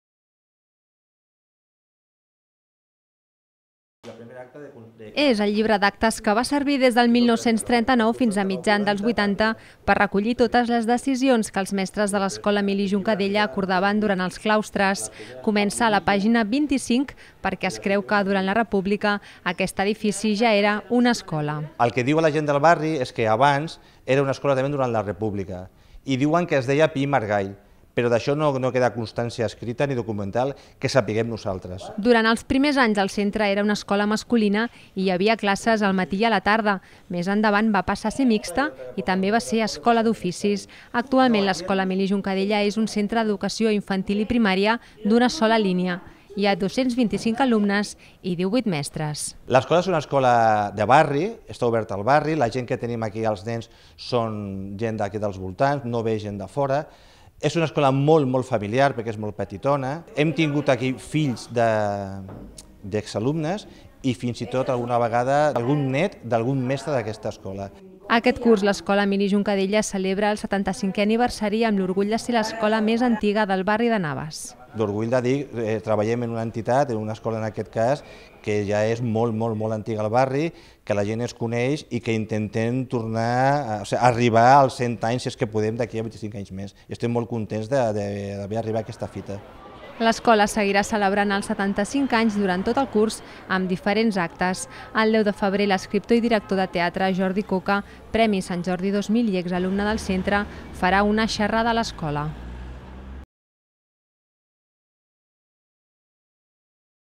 75 anys de l'escola Emili Juncadella
L'escola Emili Juncadella, de Navas, està d'enhorabona. Aquest curs escolar celebra 75 anys. De fet, el primer document de l'escola, una acta del primer claustre del professorat, data d'octubre de 1939. A més, tot i que la direcció del centre no té constància documentada, es creu que en la Segona República ja funcionava com a centre educatiu sota el nom de Pi i Margall. L'equipament, situat sempre en el mateix emplaçament, està molt arrelat al barri degut a la seva llarga tradició educativa. Més informació